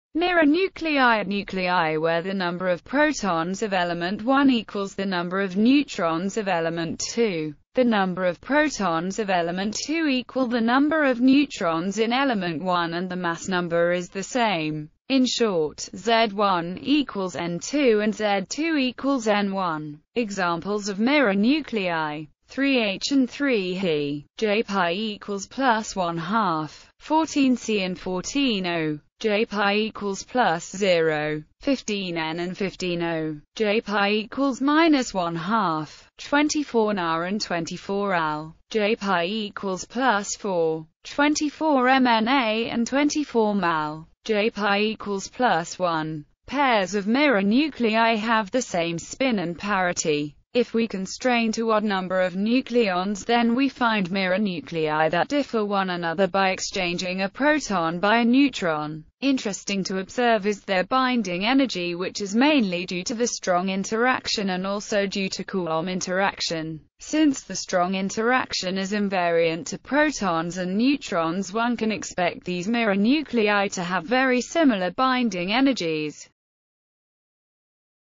Mirror nuclei
Mirror nuclei are nuclei where the number of protons of element one (Z1) equals the number of neutrons of element two (N2), the number of protons of element two (Z2) equal the number of neutrons in element one (N1) and the mass number is the same.In short, Z1 = N2 and Z2 = N1.

Image-Copyright-Info
Author-Info:   Michalsmid

Image-Copyright-Info